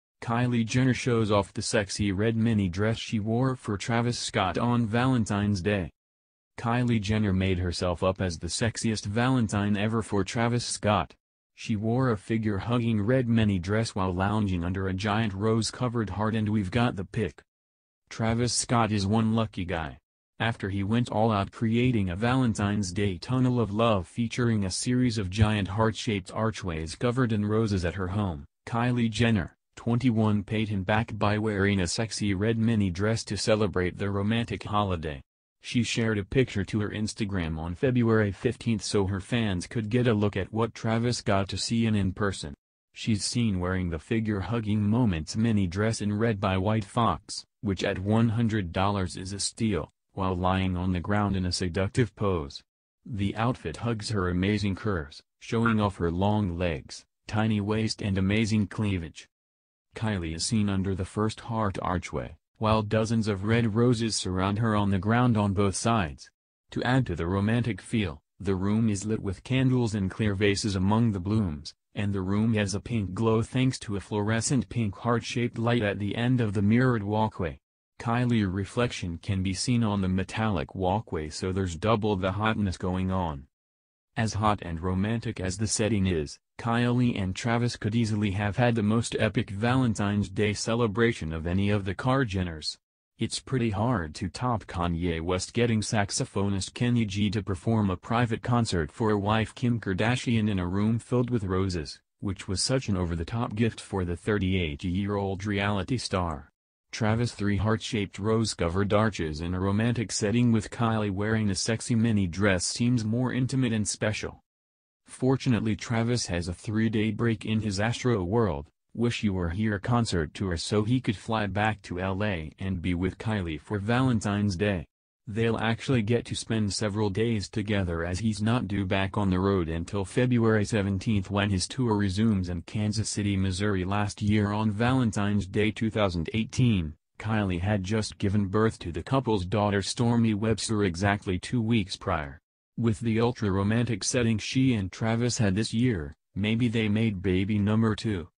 Kylie Jenner made herself up as the glamor Valentine ever for Travis Scott in a photo
Kylie Jenner Shows Off The Sexy Red Mini Dress She Wore For Travis Scott On Valentine’s Day